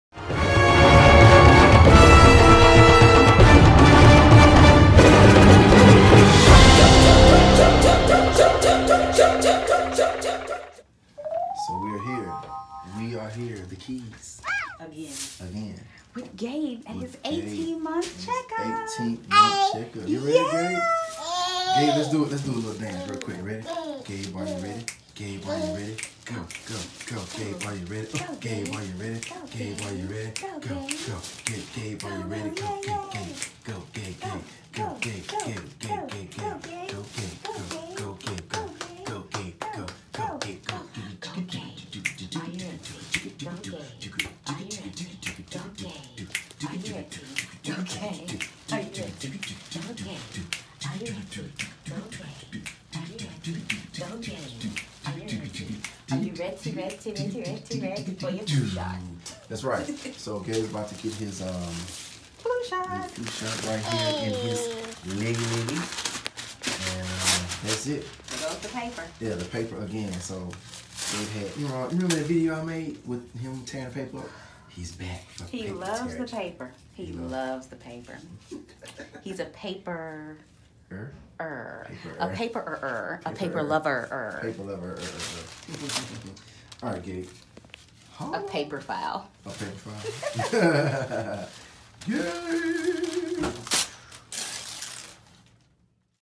Gabe’s Dr. Visit: 18 Month Check-Up
Watch as Gabe dances his way into his 18 month check up following the flu shot. He seems to be very excited. We hope this excitement last long enough for him to be at ease with his shot.